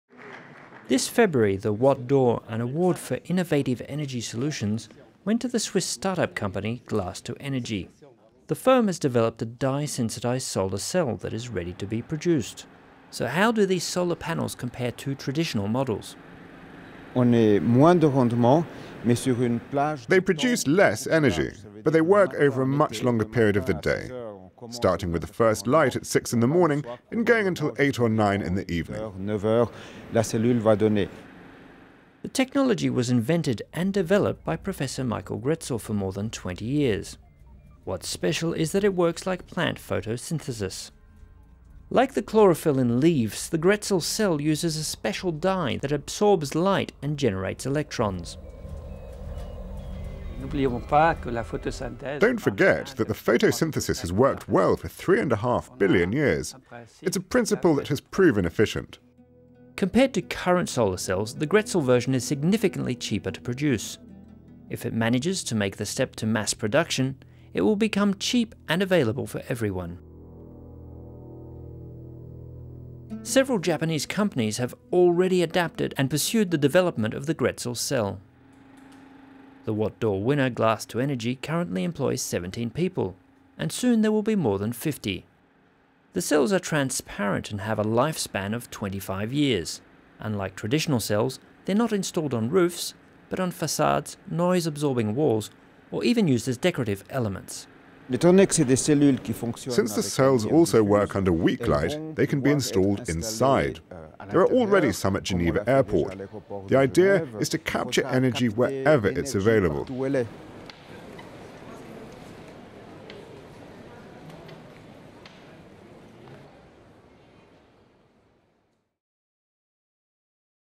Low light solar cell
This year is looking bright in Switzerland, at least as far as an energy policy is concerned. New measures introduced by the government in 2014 are giving a fresh boost to an industry that has lagged behind its counterparts in other countries. (RTS/swissinfo.ch)